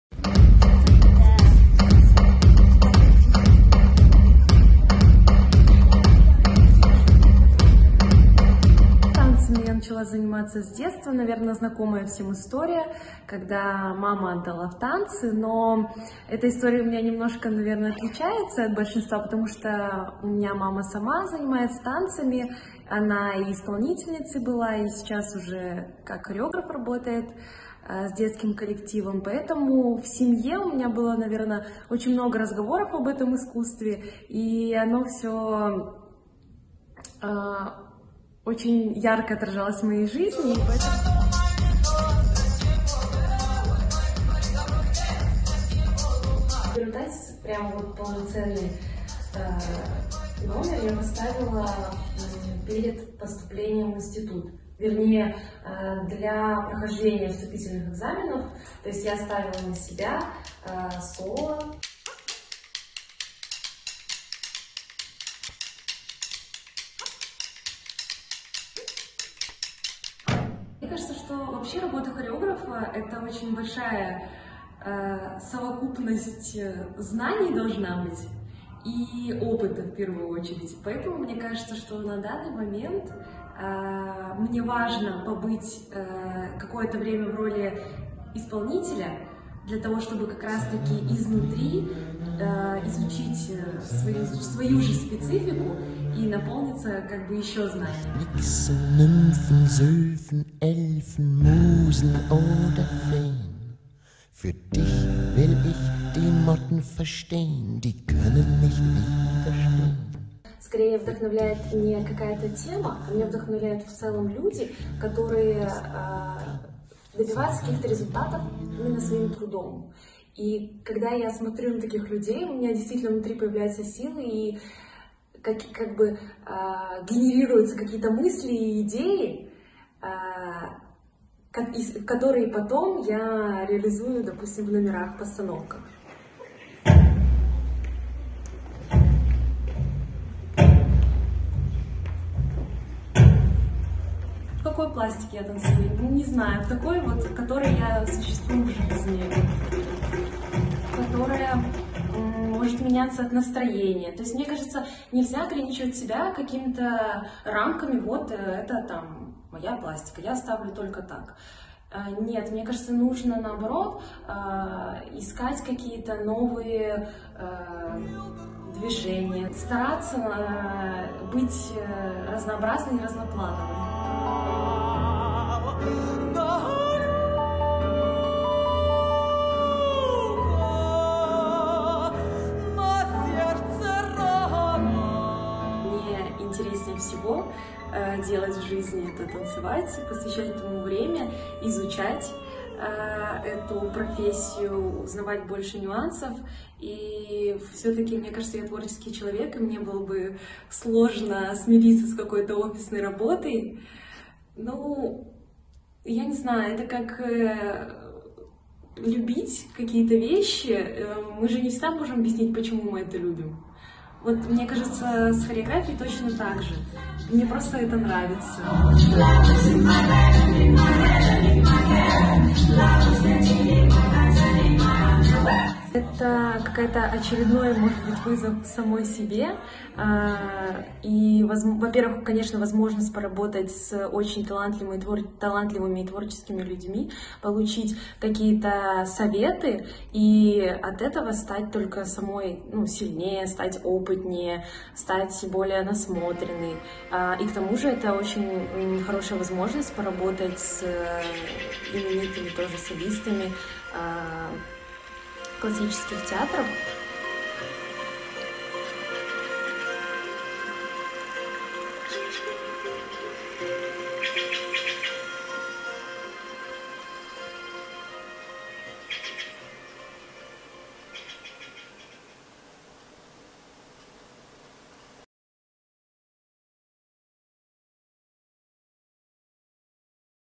Елизавета Мазуркевич, участница мастерской молодых хореографов в проекте «Русский балет навсегда».
«Вдохновляют люди, которые добиваются результата своим трудом». Елизавета Мазуркевич, участница мастерской молодых хореографов в проекте «Русский балет навсегда».